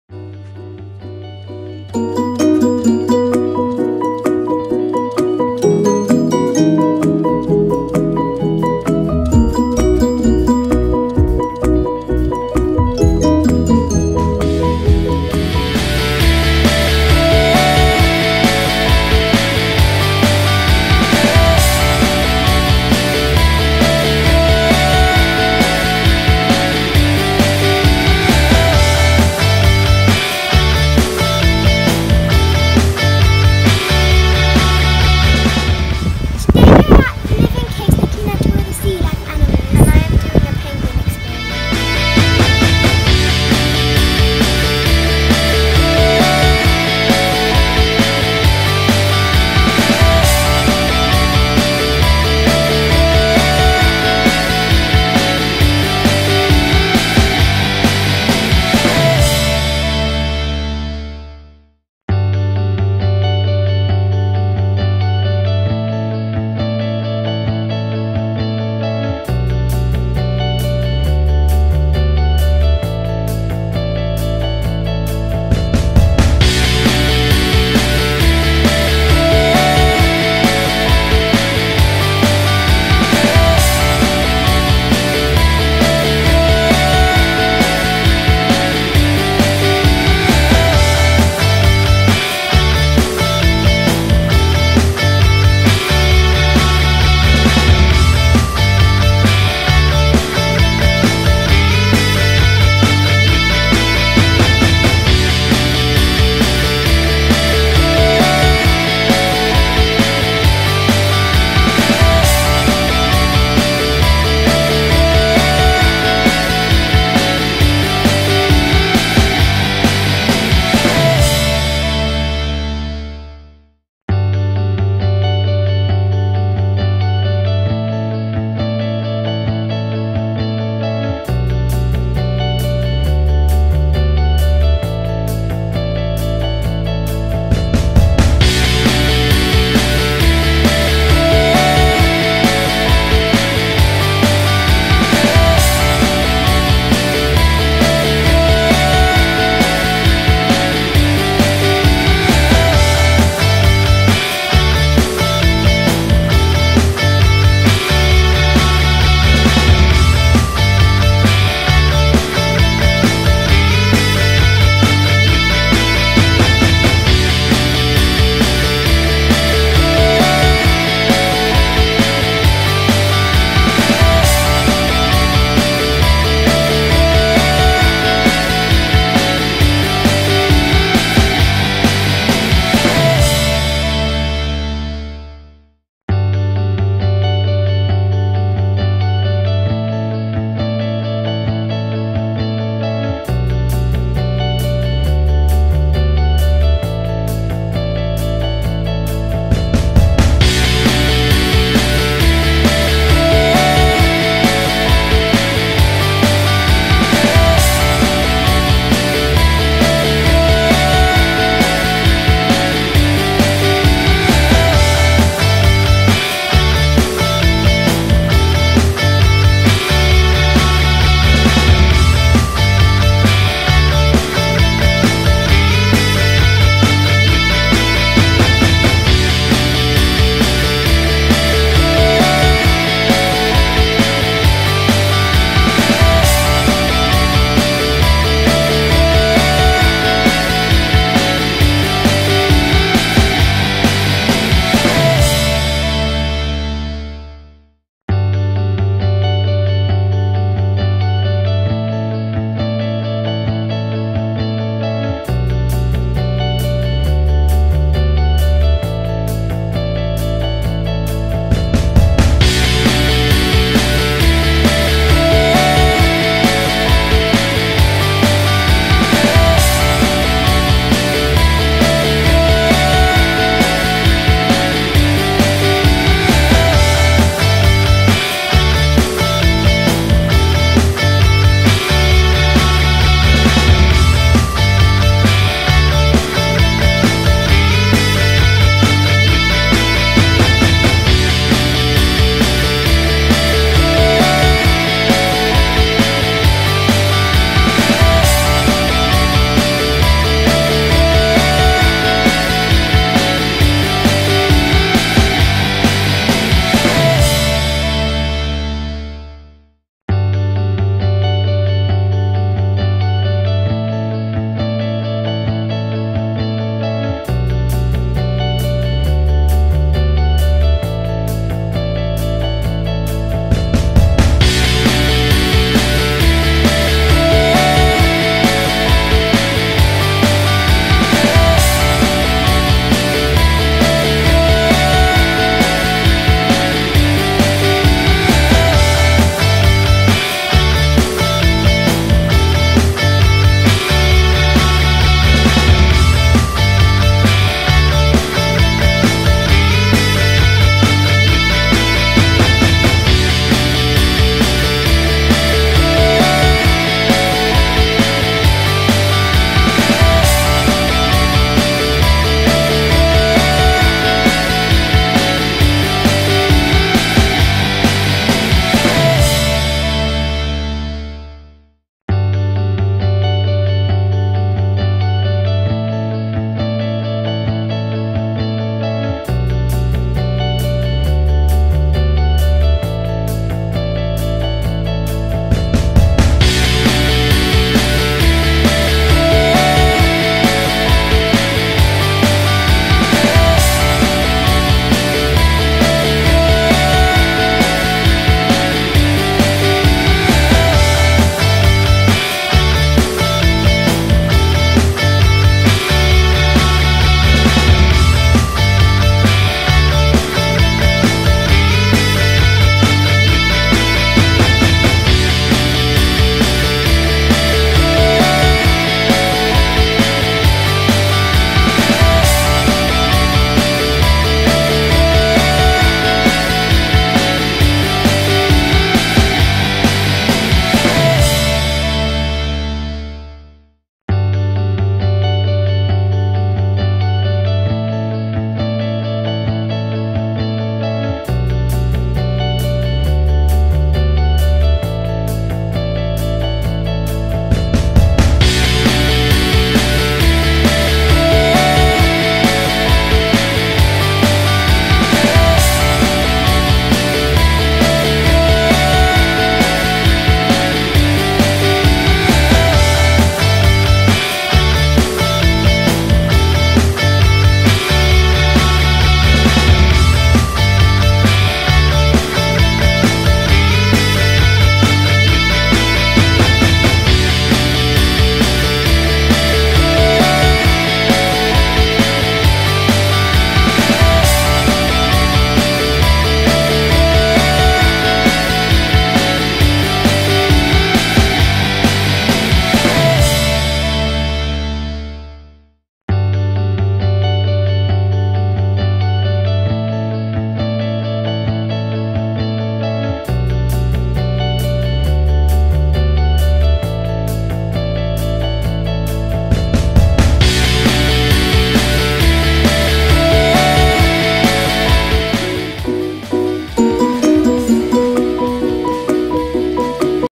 Vlog | Living Coasts & Penguin experience
All of the sea life at the Living Coasts and Ella getting to feed her favourite animal in a penguin experience!